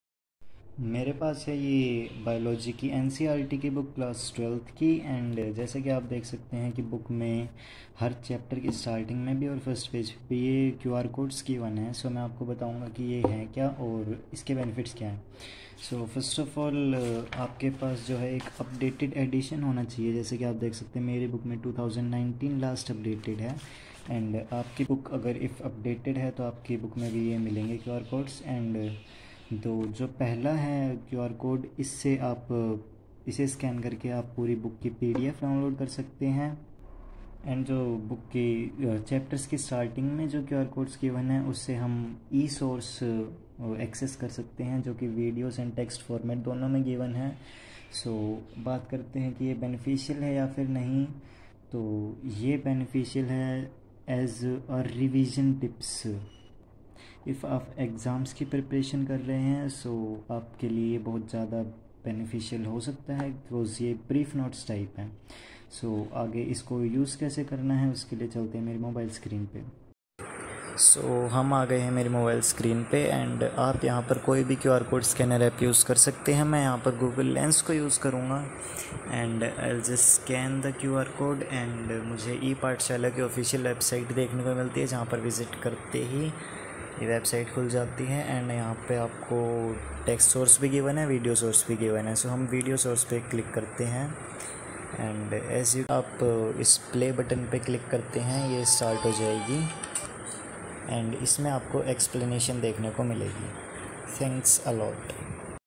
What are QR codes in NCERT books | How to use them | Are they beneficial? | GRK
I'll tell you in this video, what are these QR codes and what is their purpose? I'll give you info about this.